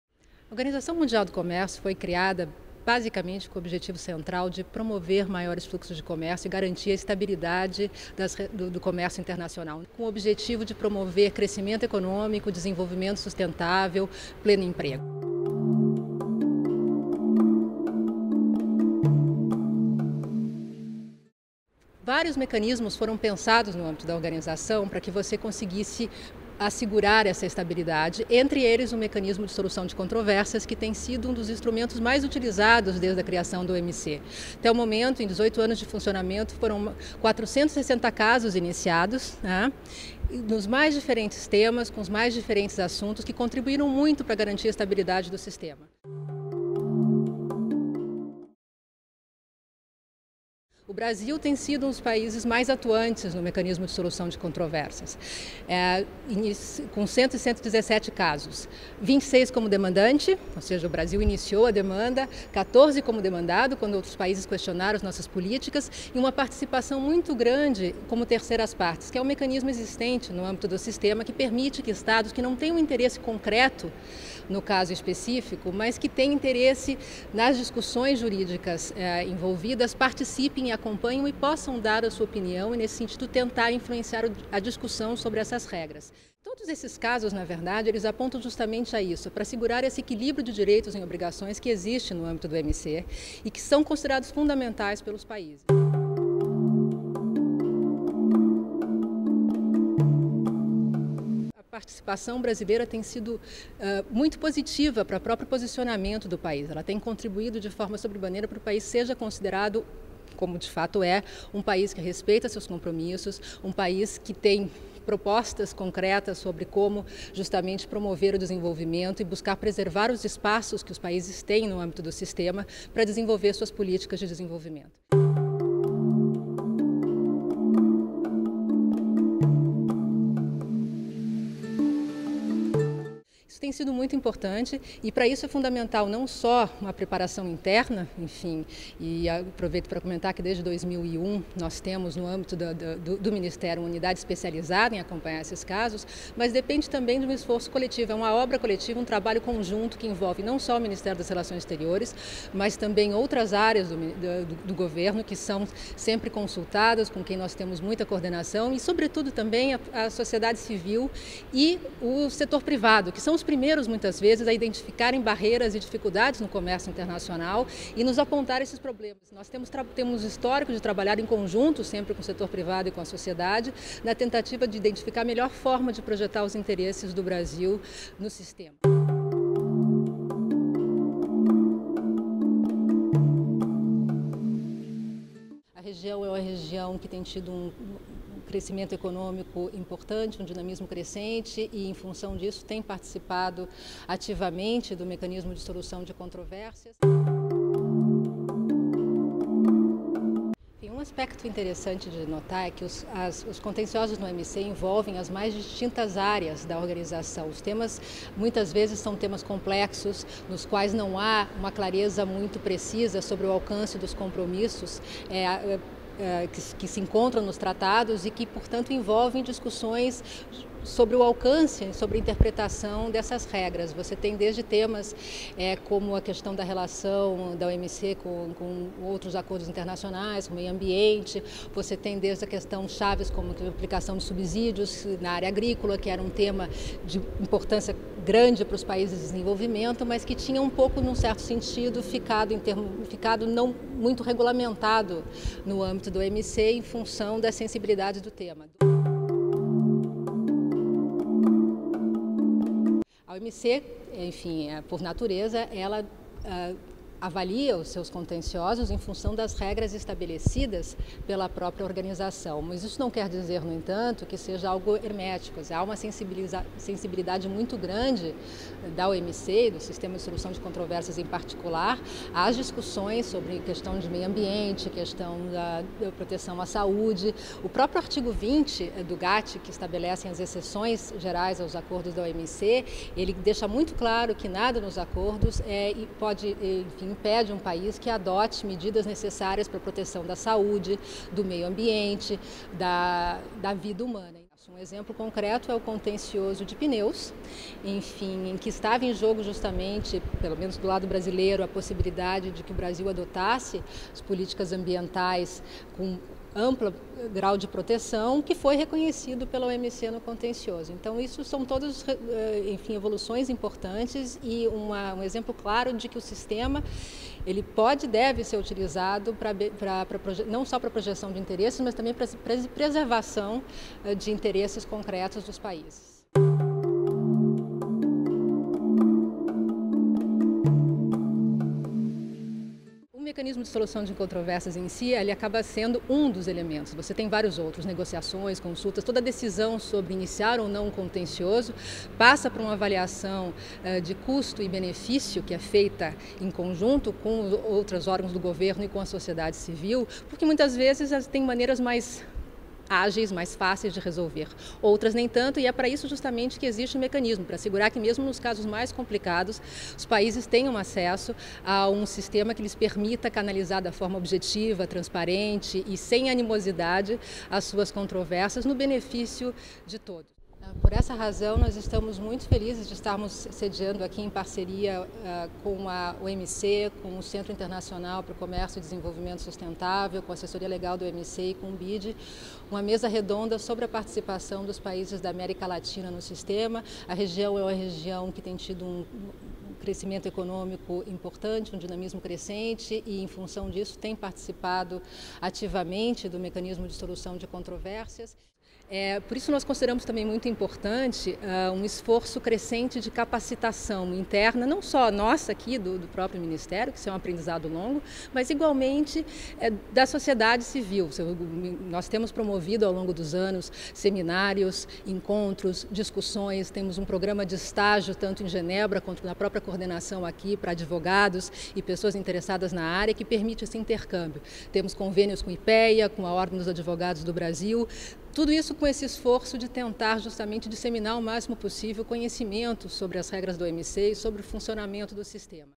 Sistema de solução de controvérsias da OMC - Cons. Daniela Benjamin
A Conselheira Daniela Benjamin, Coordenadora-Geral de Contenciosos do Itamaraty, fala a respeito da atuação brasileira no sistema de solução de controvérsias da Organização Mundial do Comércio e responde a perguntas enviadas por meio do canal Diplomacia Pública do Itamaraty.

Brasília, 9 de outubro de 2013.